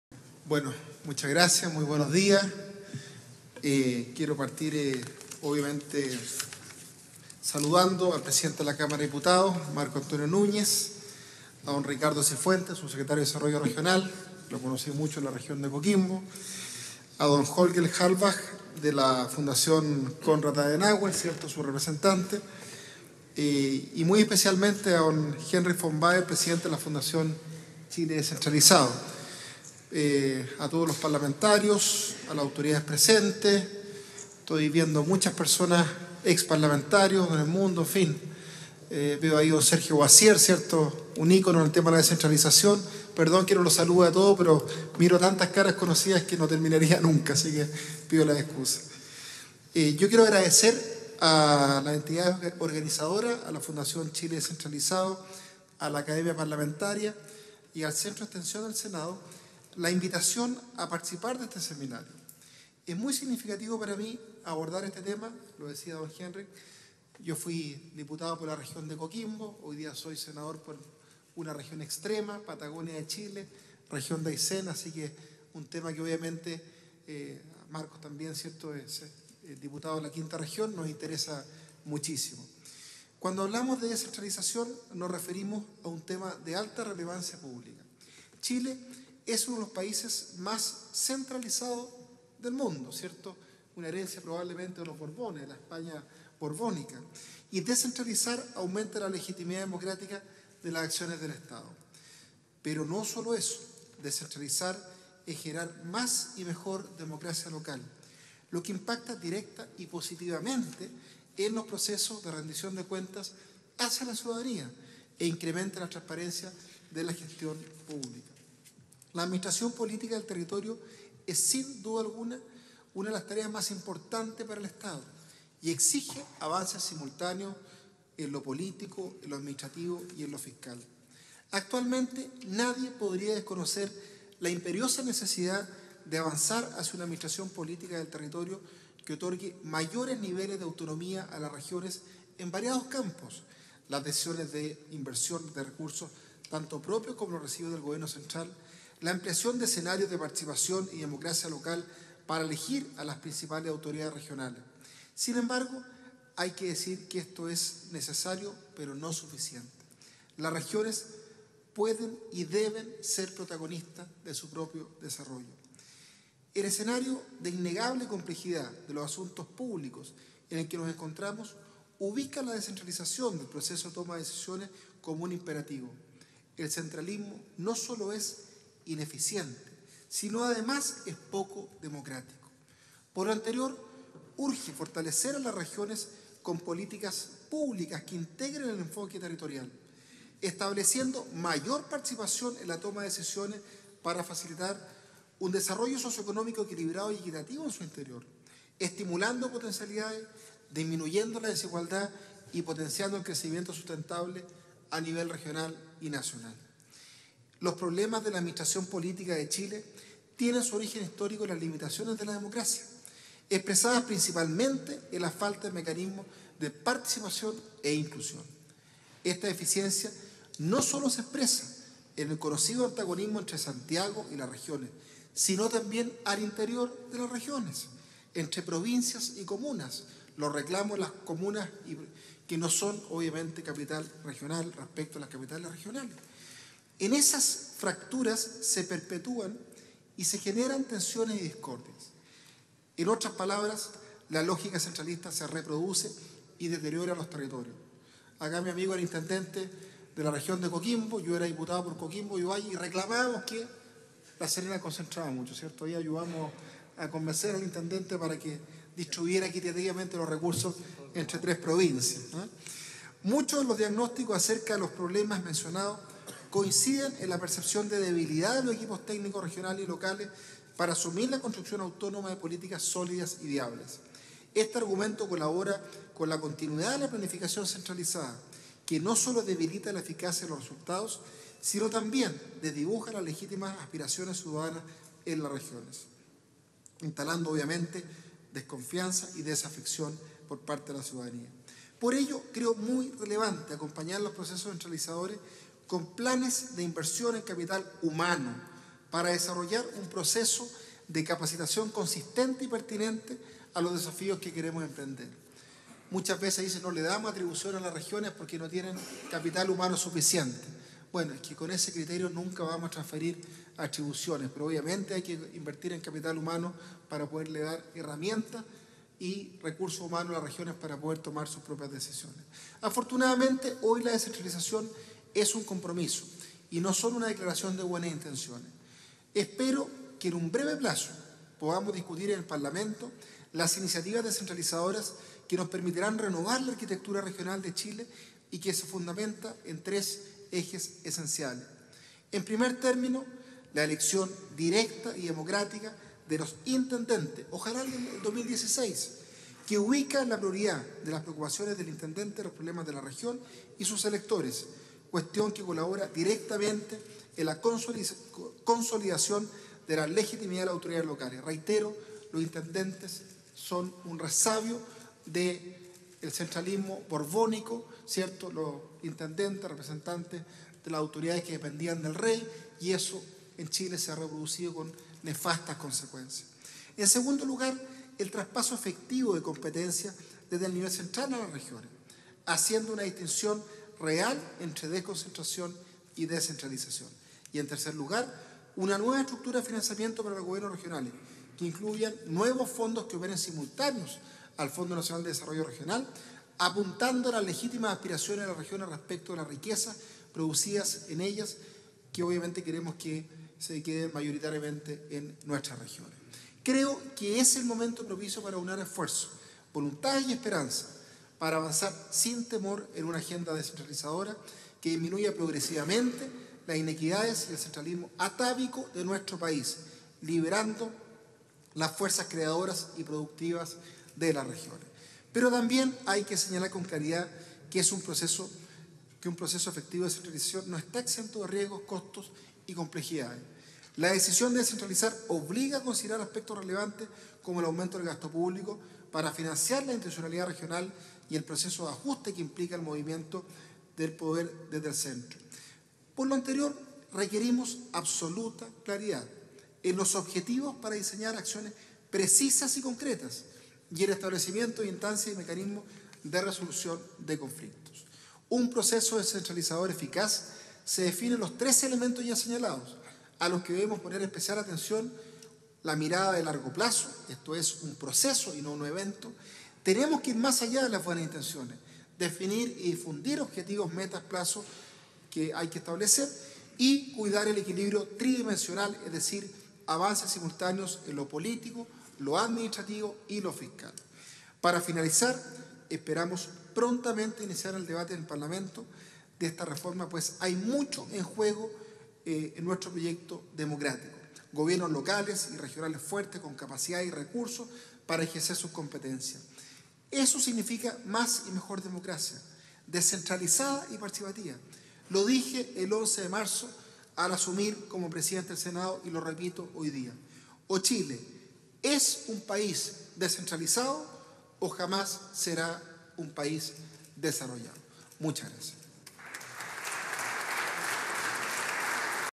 Patricio Walker Prieto. Presidente del Senado
Seminario Nacional "Marco Constitucional y Político para la Descentralización"
El Presidente del Senado indica que la lógica centralista nacional, que se reproduce al interior de las propias regiones, hace que sea necesario el aumento y traspaso efectivo de recursos, así como inversión en capacitación para un proceso de descentralización eficaz.